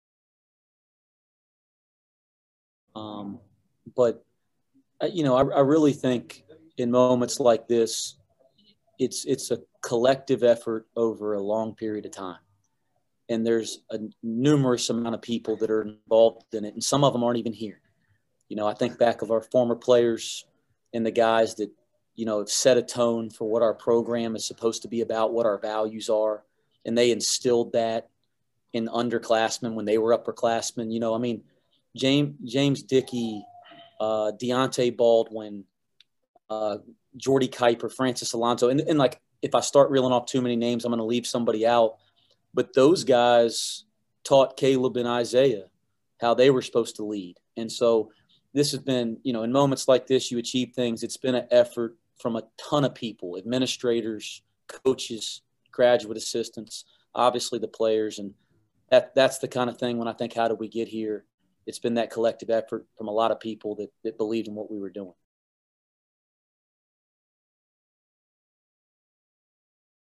Watch now: UNCG basketball coach Wes Miller discusses his program
Coach Wes Miller discusses the building of his program at UNCG.